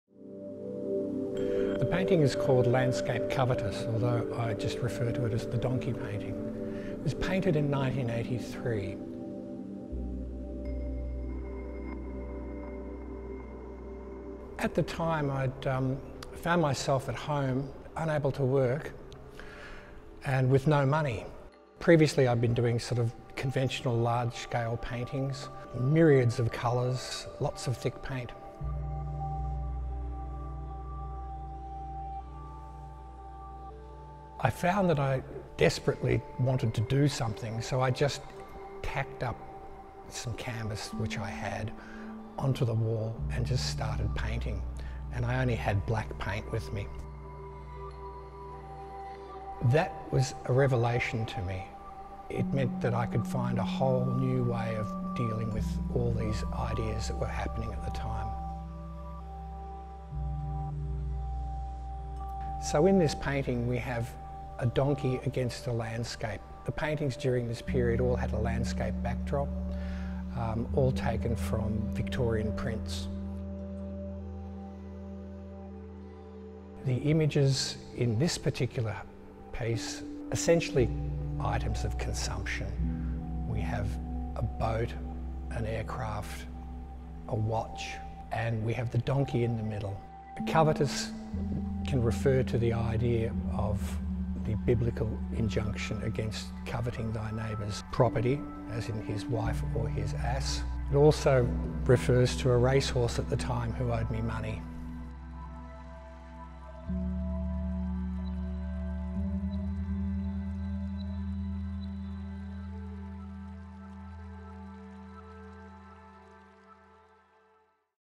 Steig Persson on "Landscape (Covetous)"
Stieg Persson talks about his artwork "Landscape (Covetous)" as part of his exhibition, "Polyphonic" at the Potter Museum of Art.